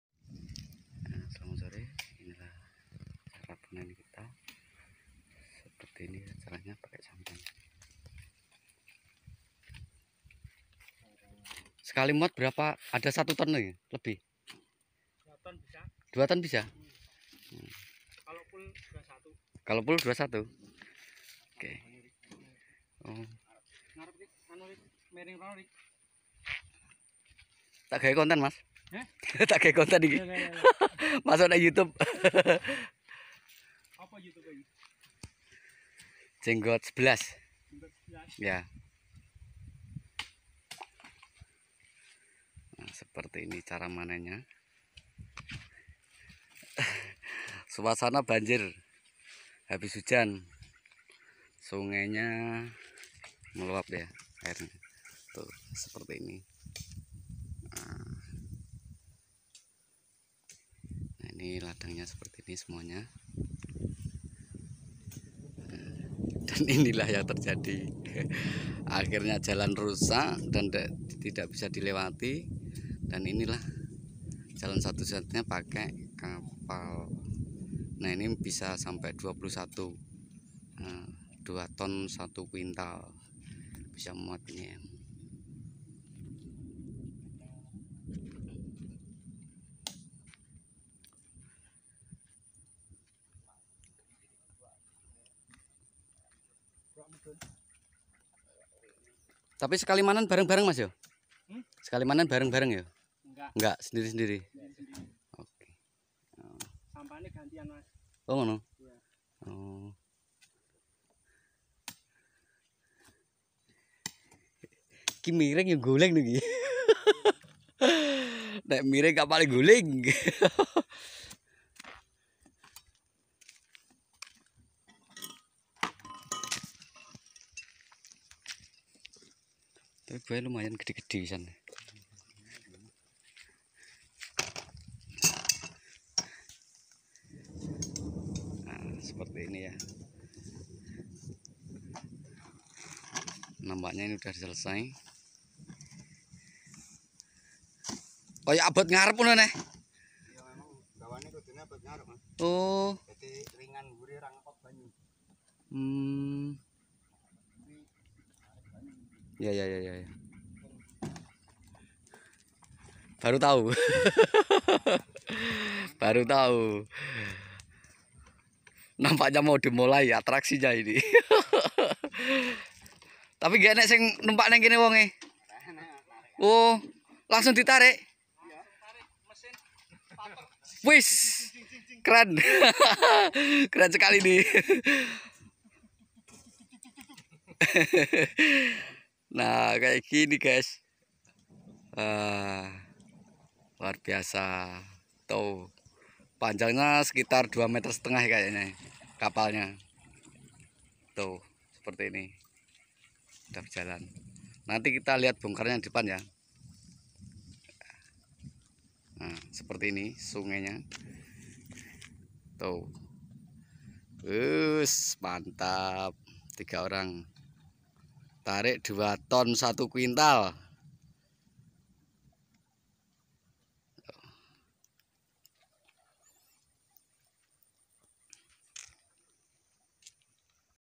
Petani kreatif bikin kapal untuk langsir sawit sekali angkut 2 ton.
Petani kreatif bikin kapal untuk langsir sawit